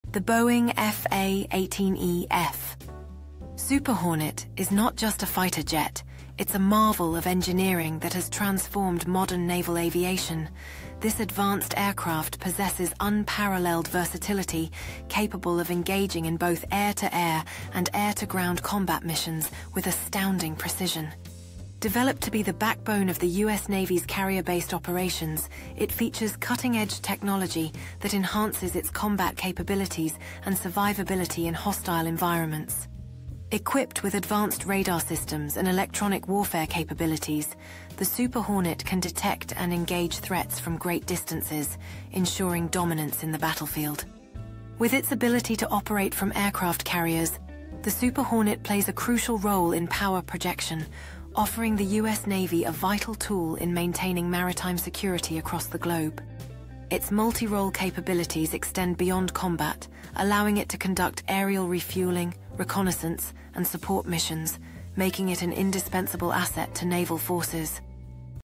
F/A-18E/F Super Hornet. Super Hornet jet carrier-multi-role fighter capable of extended flight
Discover the Boeing F/A-18E/F Super Hornet, a pinnacle of modern naval aviation that redefines combat versatility. From its breathtaking maneuvers to cutting-edge technology in its cockpit, this aircraft exemplifies engineering excellence.

Join us as we witness the Super Hornet's operations on aircraft carriers, showcasing its crucial role in maritime security and power projection. You'll see it in action during aerial refueling and high-speed maneuvers, proving its multifaceted capabilities beyond just air-to-air combat.

This video highlights why the Super Hornet remains an indispensable asset to the U. S. Navy.